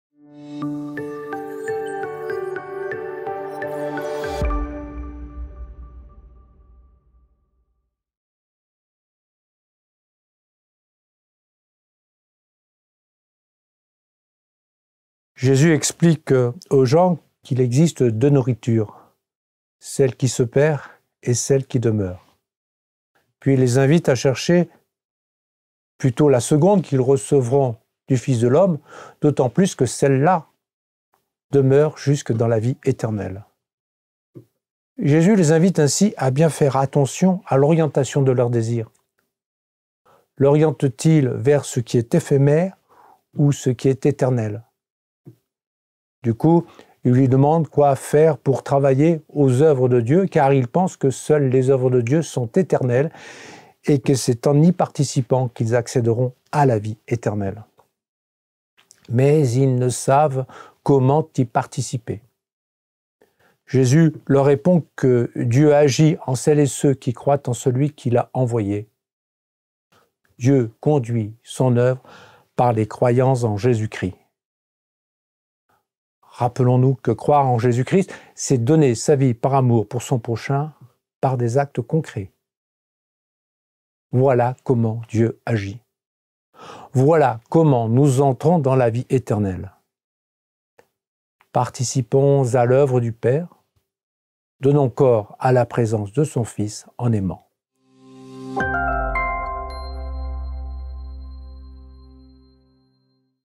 [ 𝑳𝒂 𝑴𝒊𝒏𝒖𝒕𝒆 𝑺𝒑𝒊 🙏 ] lundi 19 avril : Jean 6, 22-29
« Travaillez non pas pour la nourriture qui se perd, mais pour la nourriture qui demeure jusque dans la vie éternelle »

Lundi 19 avril  : Jean 6, 22-29 avec le père Olivier PETIT, curé de la paroisse Notre-Dame de Vie.